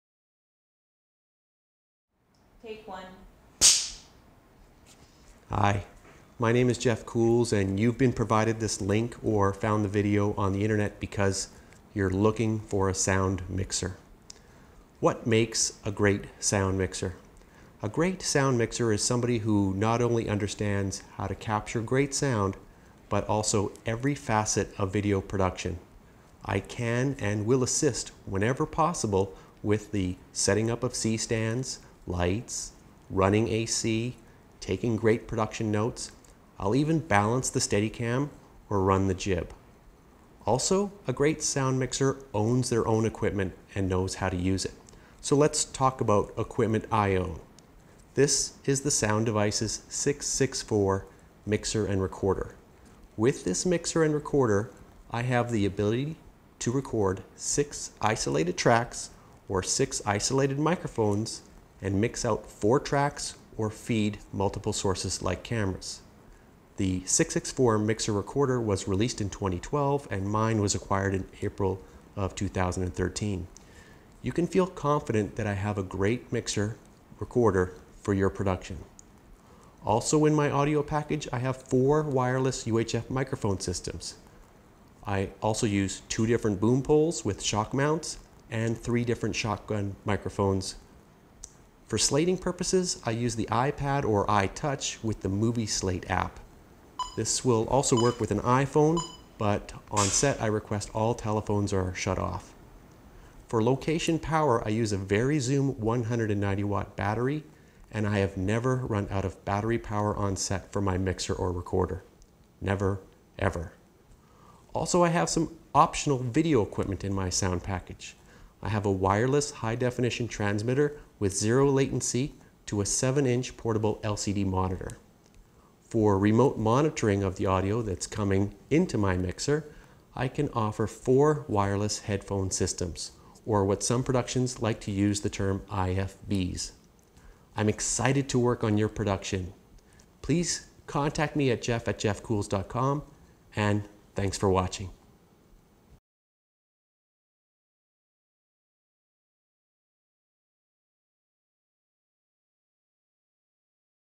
On Location Sound Mixing for TV and Film Video Projects in Scottsdale Arizona, USA
Jeff Cools is a sound mixer in Peoria, AZ and records audio for TV and film and video  projects on-location. He owns all of his gear: Sound Devices 664 recorder/mixer, 7 wireless microphones, 4 wireless headsets for director/client monitoring, boom poles, shotgun mics and dialog mics.

Contact Jeff Cools today for your location recording!

Have gear and experience...will Travel!